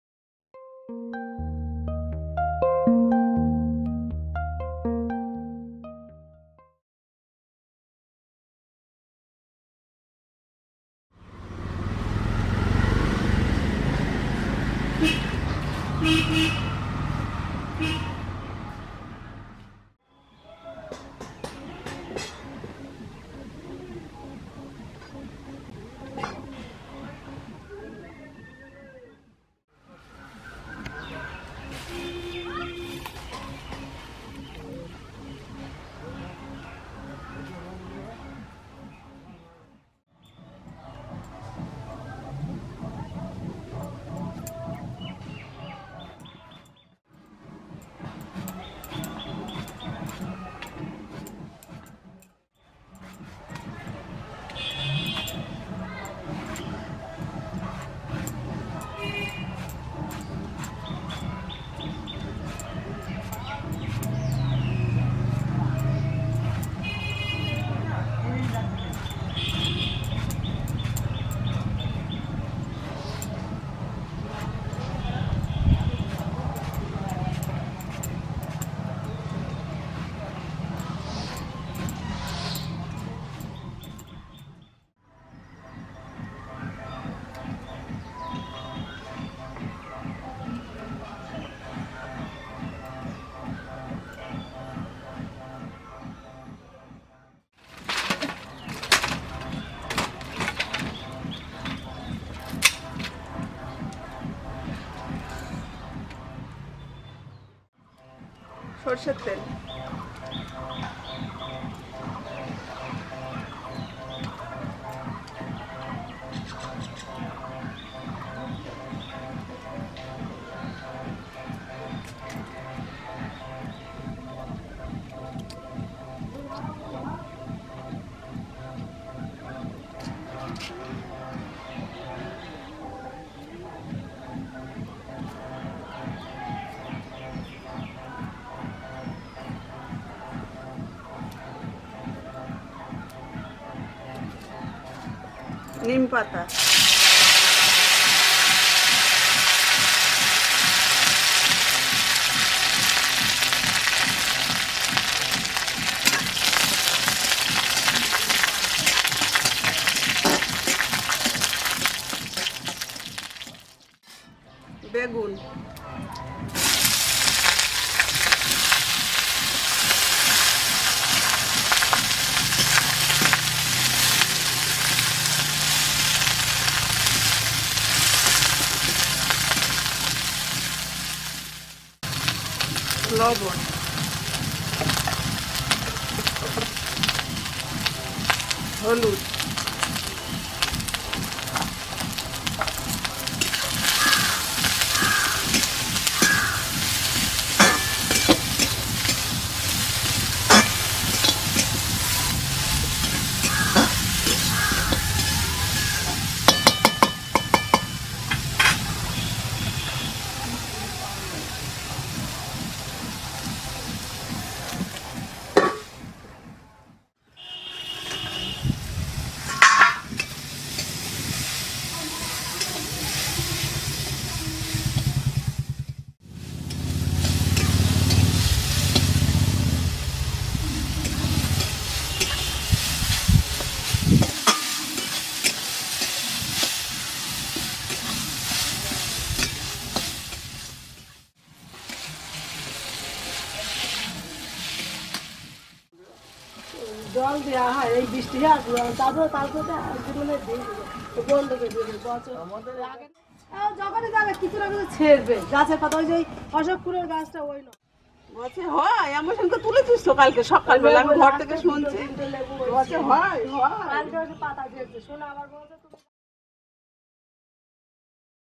গরমে মুখে রুচি আনতে সহজেই বানানো নিম বেগুনের এই রেসিপি || Healthy Recipe
1.Neem Leaf
2.Eggplant/बैंगन
3.Mustard oil/सरसों का तेल
4.Turmeric powder/हल्दी पाउडर
5.Salt/नमक